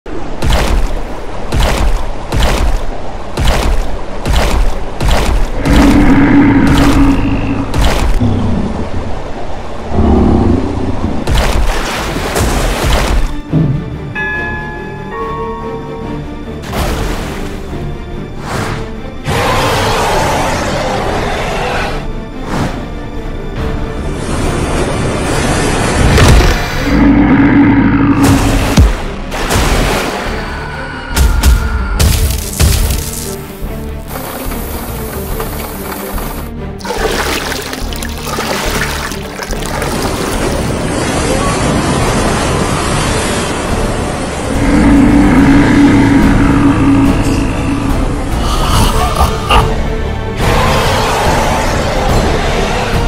Day 18 vs SCP-3700-2 | Monster Animation
Day 18 walking in sea then he accidentally stomp on SCP-3700-2 who's currently sleeping. SCP-3700-2 got angry and began to attack Day 18. Day 18 doesn't feel anything and kick SCP-3700-2 away. Can SCP-3700-2 got his revenge ? Let's find out with us in this video.

Featuring Trevor:
- Day 18

Featuring SCP:
- SCP-3700-2